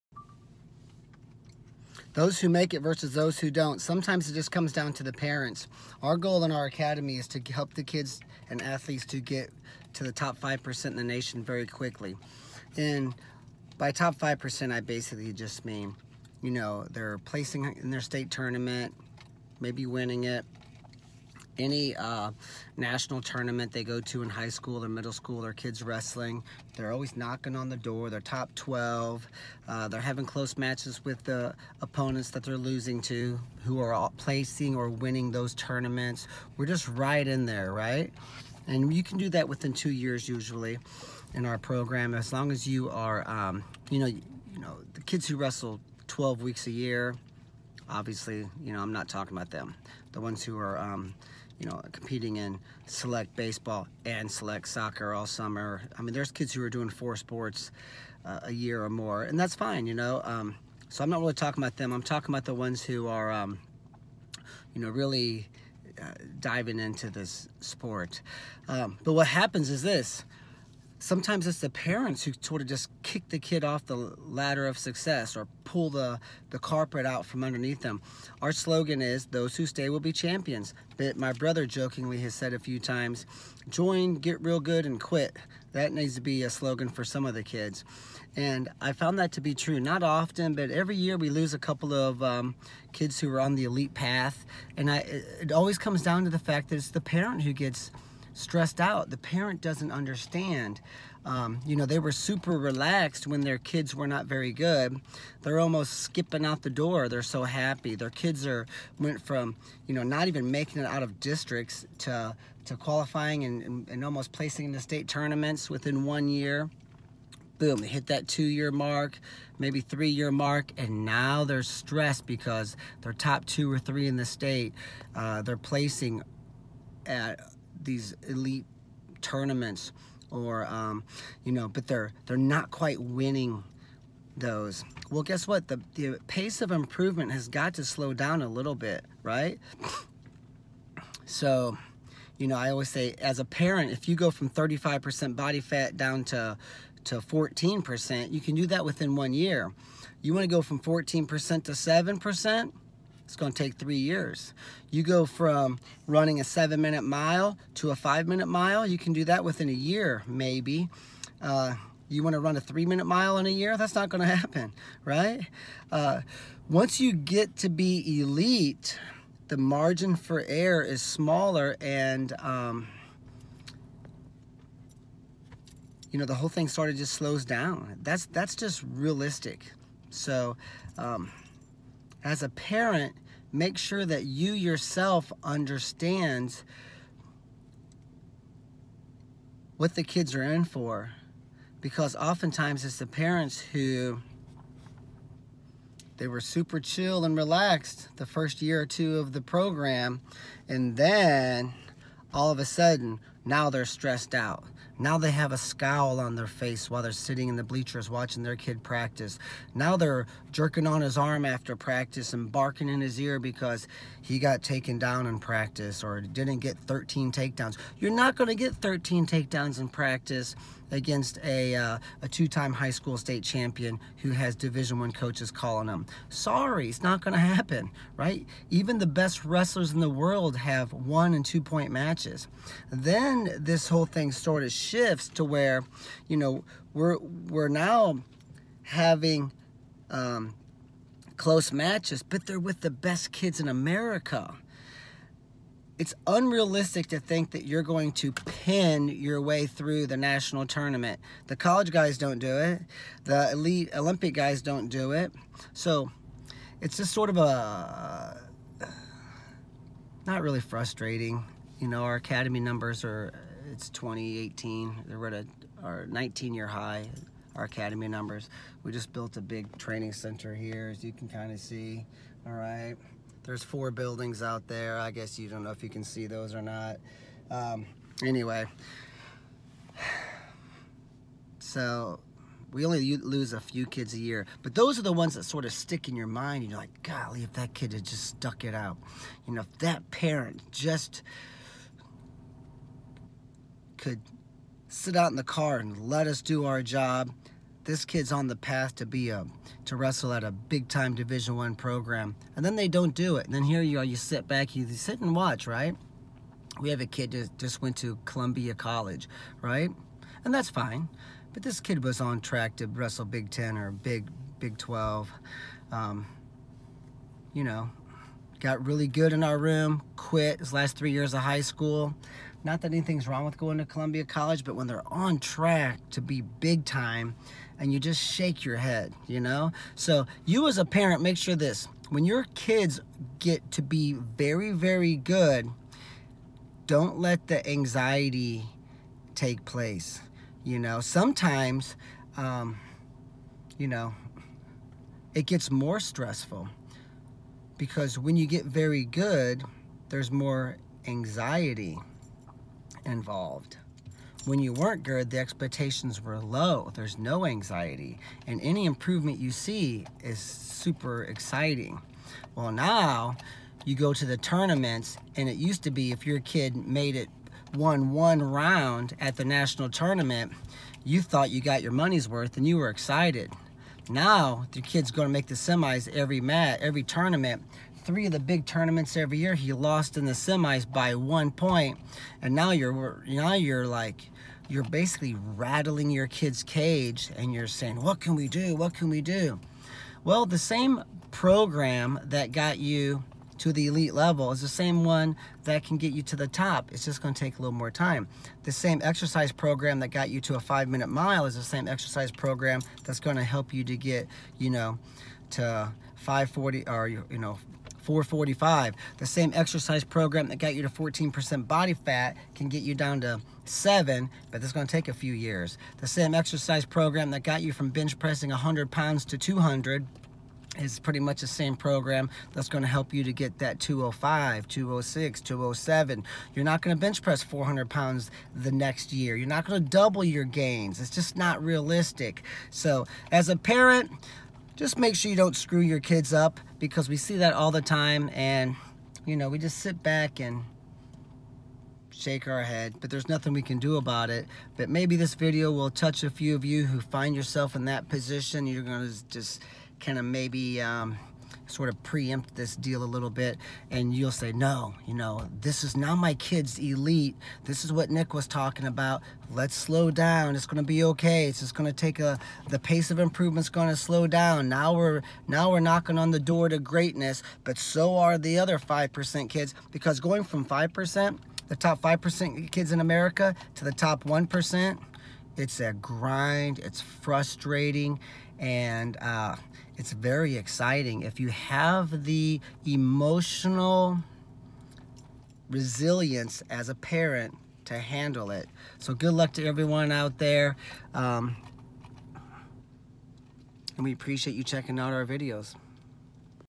It’s the PARENTS who sabotage their kids’ success ALL TOO OFTEN. Are YOU allowing YOUR stress and l
It’s the PARENTS who sabotage their kids’ success ALL TOO OFTEN. Are YOU allowing YOUR stress and lack of understanding about success at an elite level to ruin your kids’ chances???